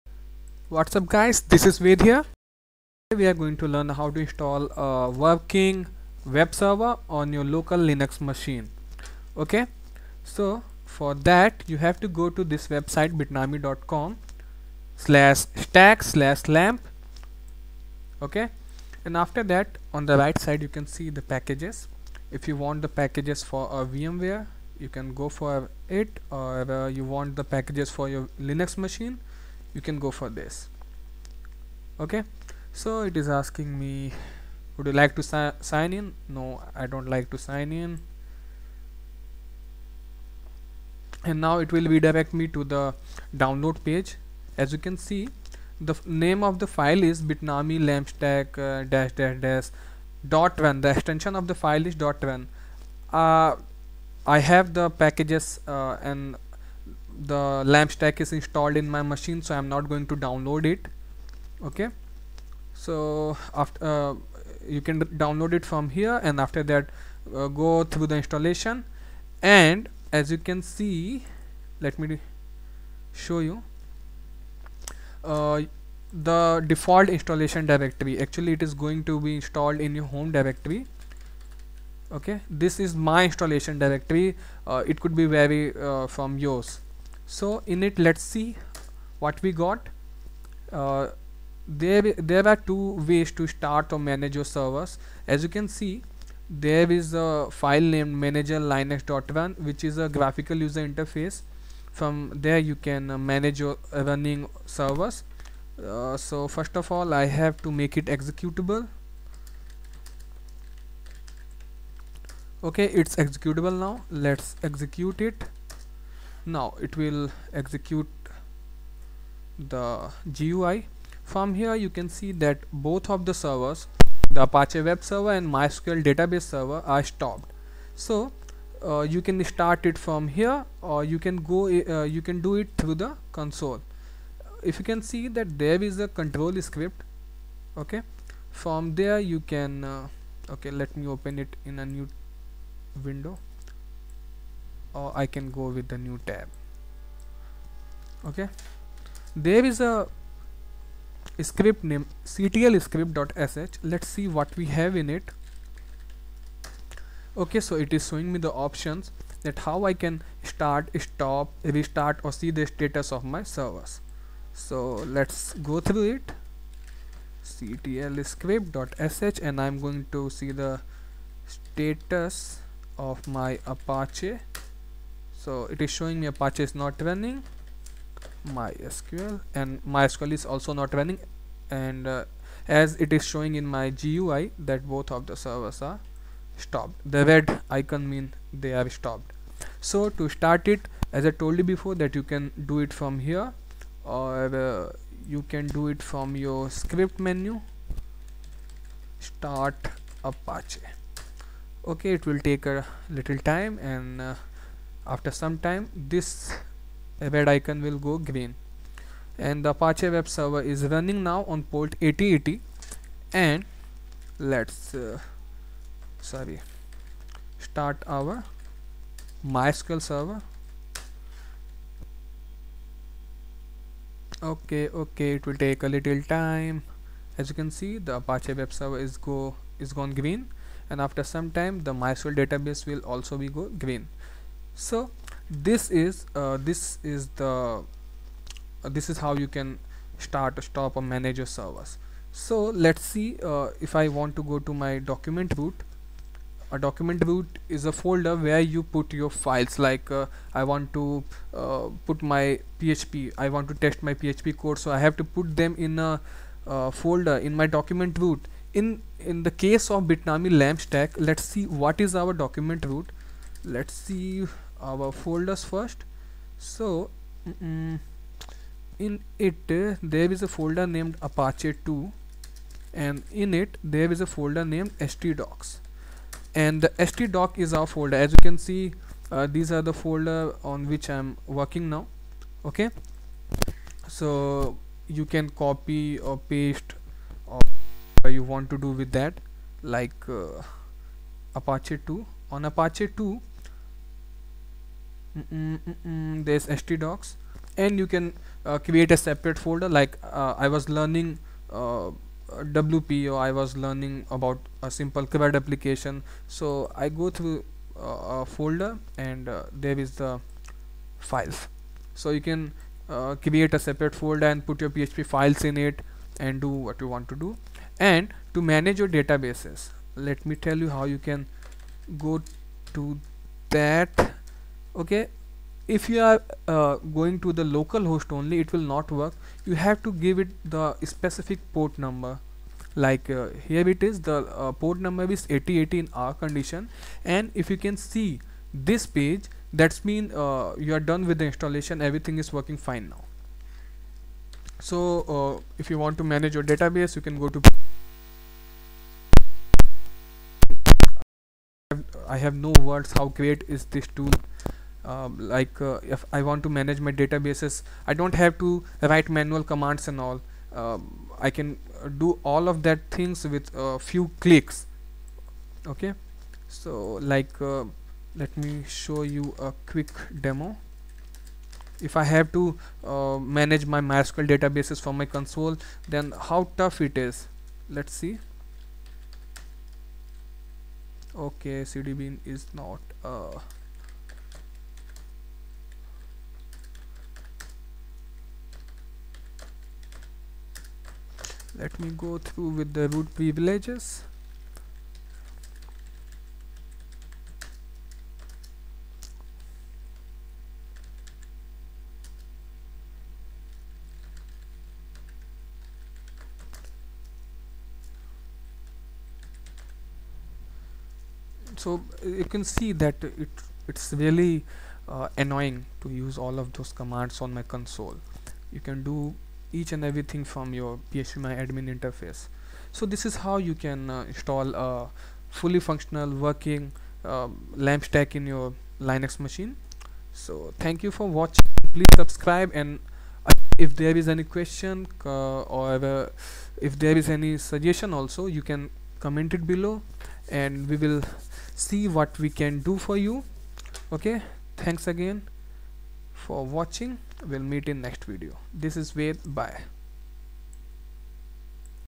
Installing LAMP Stack
In this video you are going to learn about the installation of a fully functional and working web and database server